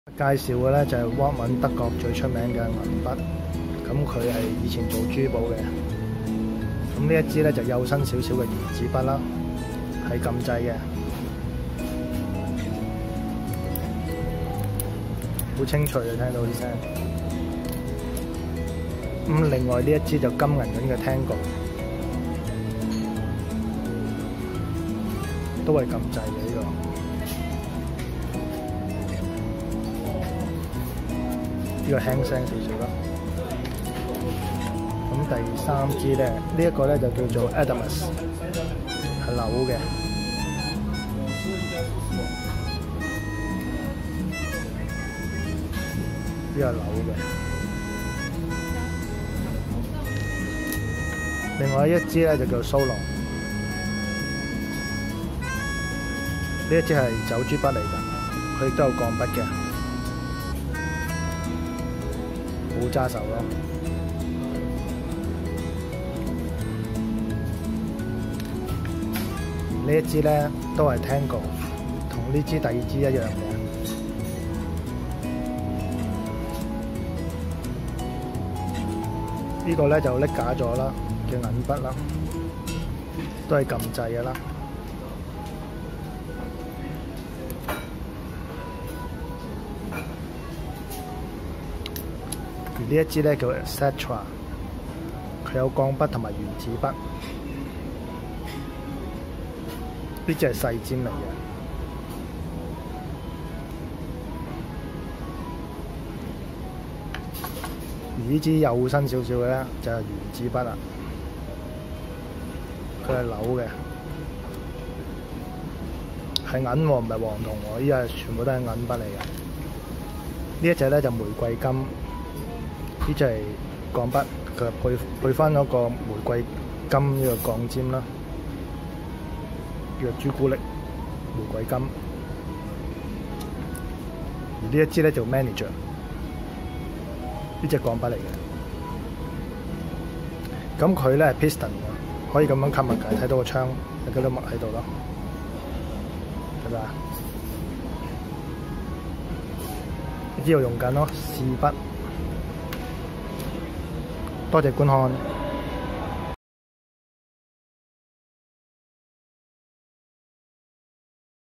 一次過了解德國品牌Waldmann純銀款式 - 廣東話開箱
Waldmann 筆系列由左至右介紹：

1. Eco Ballpen 原子筆
2. Tango Bi-color Ballpen 原子筆
3. Adamas Ballpen 原子筆
4. Solon Gel Roller 走珠筆
5. Tango Black Lacquer  Ballpen 原子筆
5. Xetra Fountain Pen 鋼筆
6. Xetra Ballpen 原子筆
7. Tuscany Fountain Pen 鋼筆
8. Manager  Piston Fountain Pen 鋼筆

1918年由Adolf Waldmann創立，創立初期以黃金和925銀製作機械鉛筆為主，秉承悠久的歷史及傳統，一直以925銀來製造優質的書寫工具。Waldmann為少數德國書寫品牌中仍堅守著百分百在德國製造的企業，所有的筆都是由內部工匠以手工打造。
送禮佳品，德國製造，高貴禮盒，925純銀筆身，十年保養